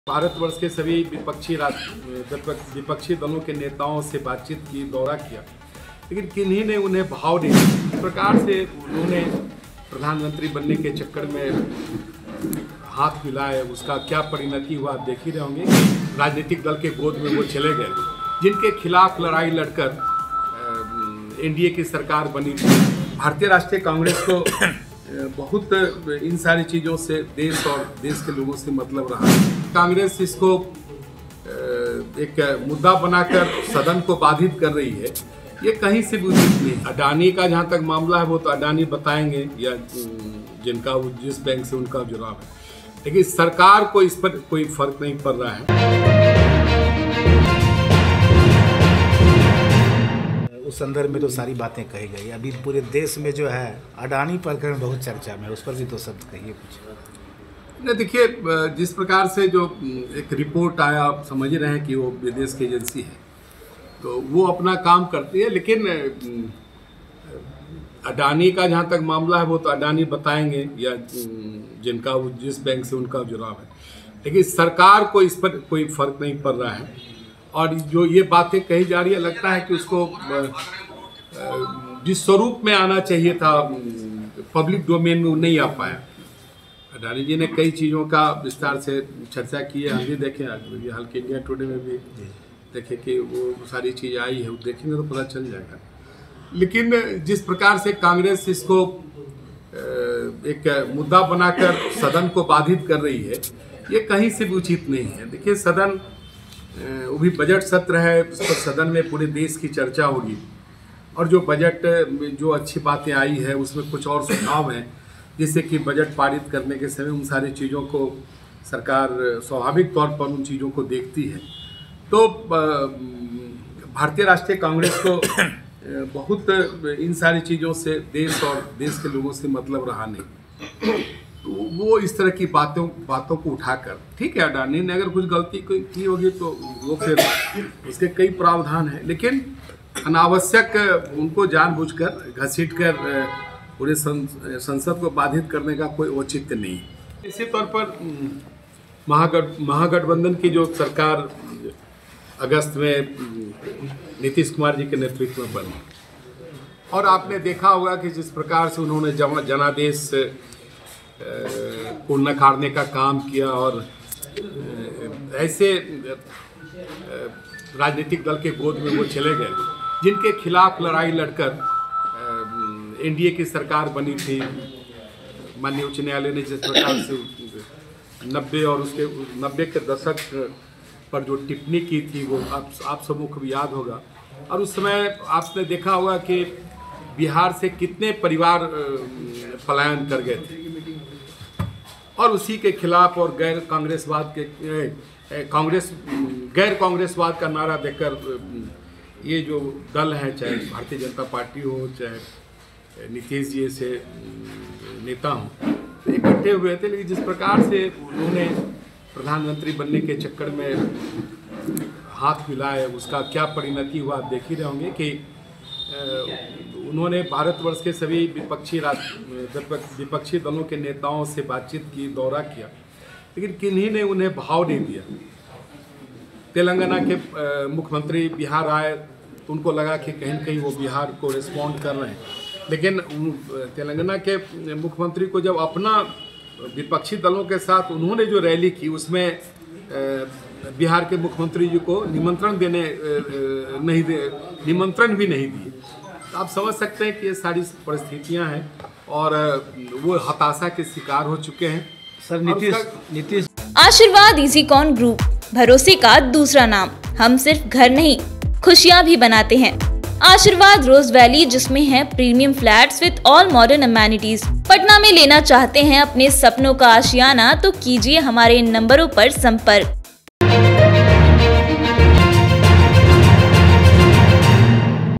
तारकिशोर प्रसाद ने कहा- अडानी के मुद्दे से भारत सरकार को नहीं पड़ता है फर्क, नीतीश जिनके खिलाफ...
अडानी के मुद्दे से भारत सरकार को नहीं पड़ता है फर्क, जिनके खिलाफ लड़ते थे उन्हीं के साथ चले गए हैं नीतीश, विपक्षी एकजुटता पर भी बोले तार किशोर प्रसाद